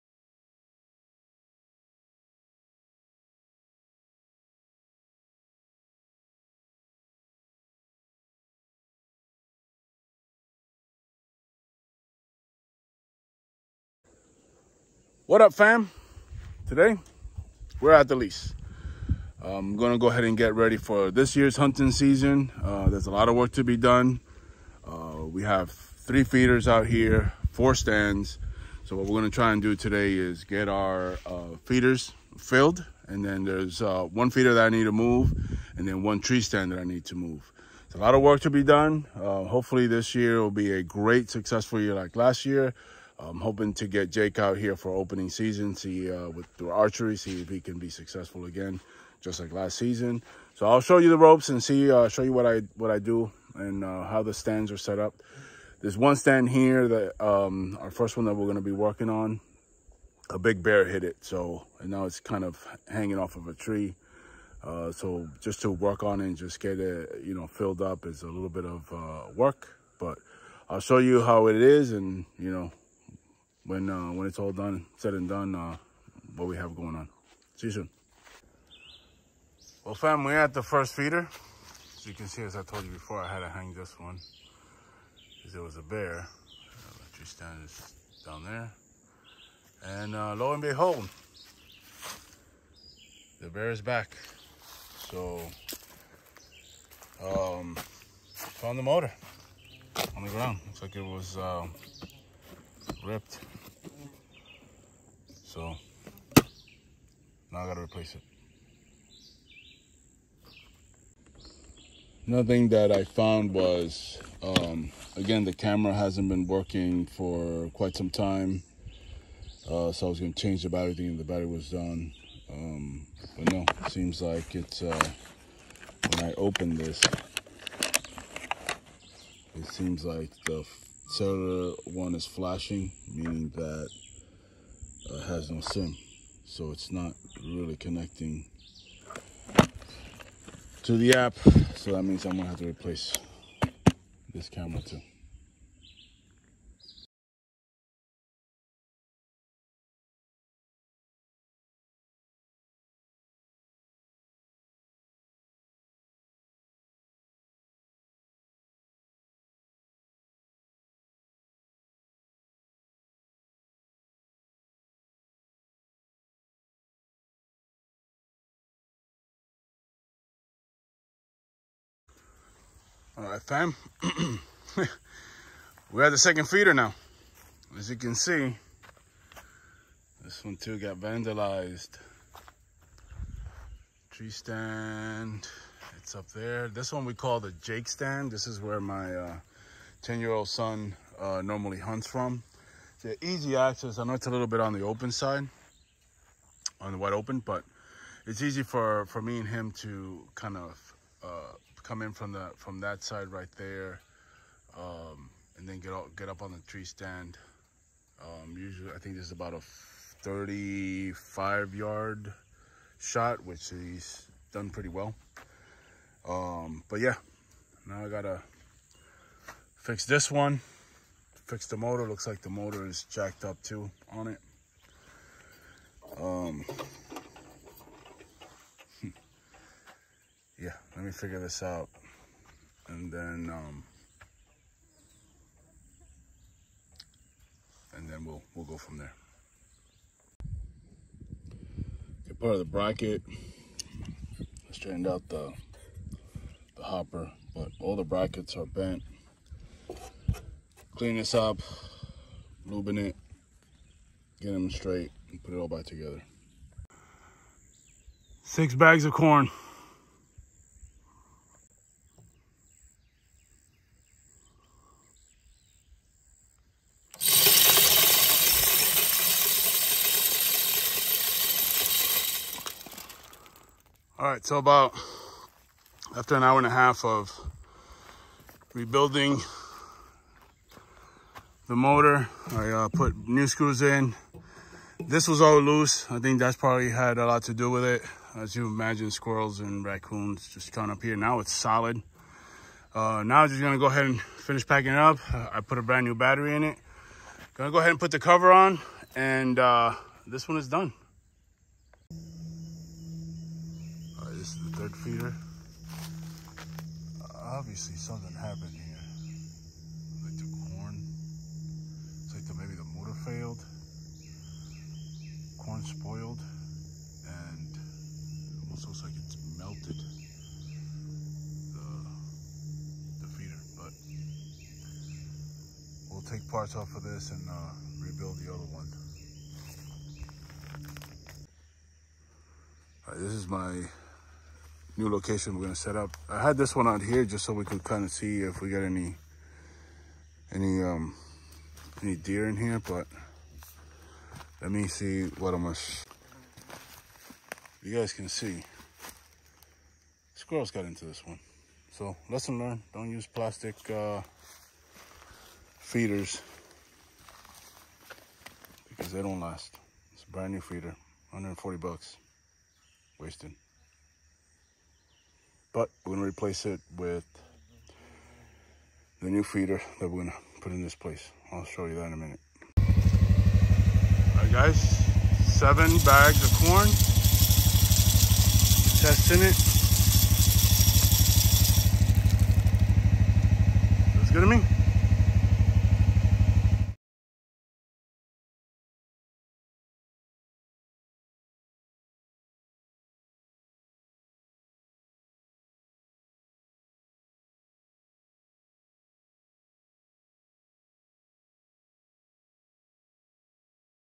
Florida Black Bear vs Hunter
Florida Black Bear attacks again!
Black Bear damaged 2 Feeders and is a problem in Florida. He damaged 2 feeder motors and another feeder got damaged beyond saving. This is what we do to get 2024 season underway.